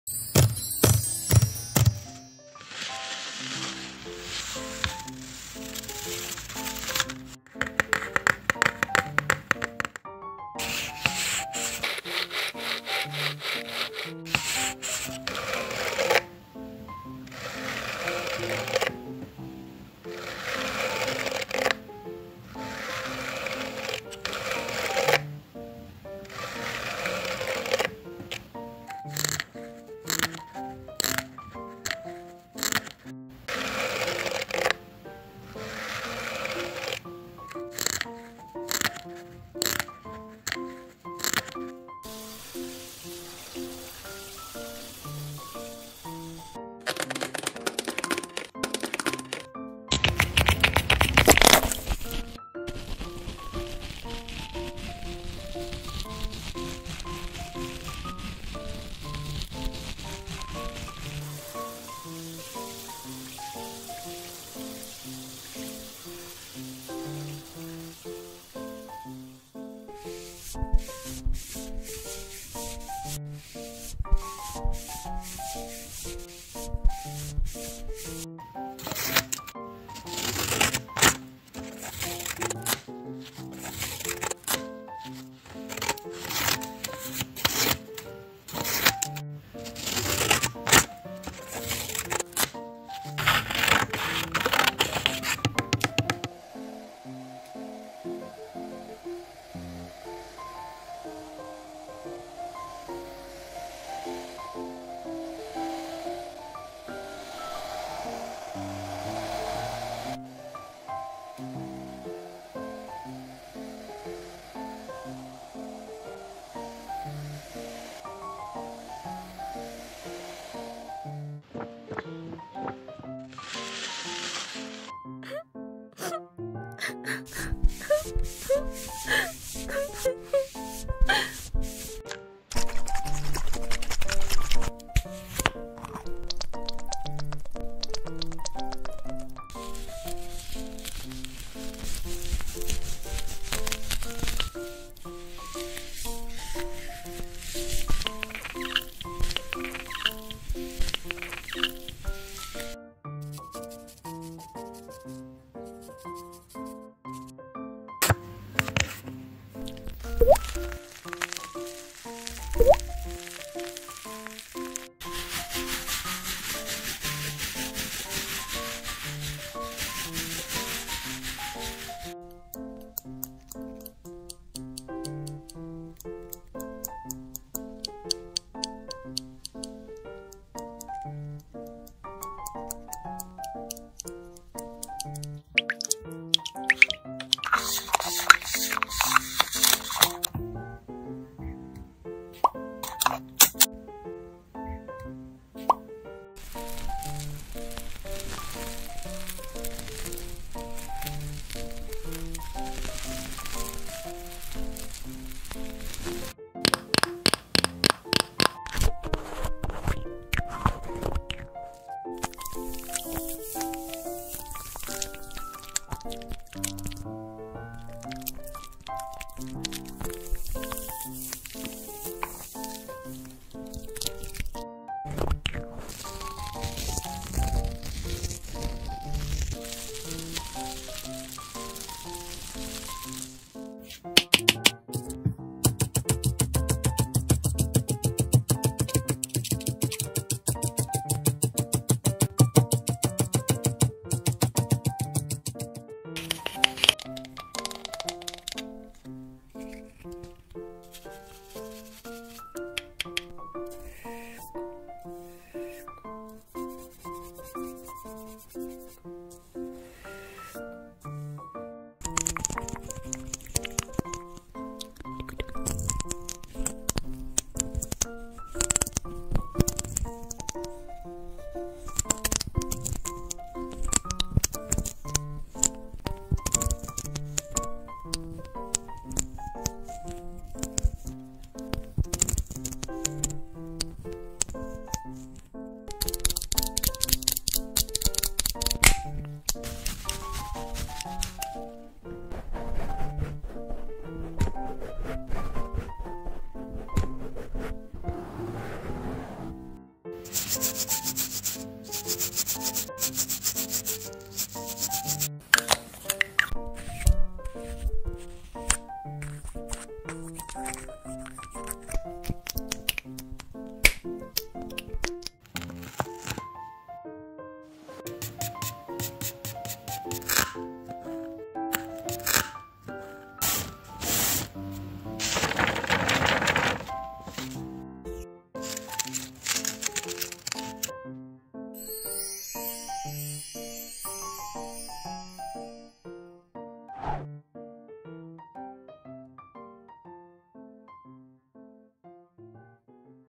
[Paperdiy] ASMR Transformation Homeless girl | With paper cosmetics 👩‍🦰✨ 🎶
HI guys! , I hope you enjoy watching the video 🥰
don't forget supporting me To continue.. thank you 🌹

ASMR transformation homeless girl with paper cosmetics💄✨
asmr makeup and skincare for homeless women with paper cosmetics ✨
asmr makeup with paper cosmetics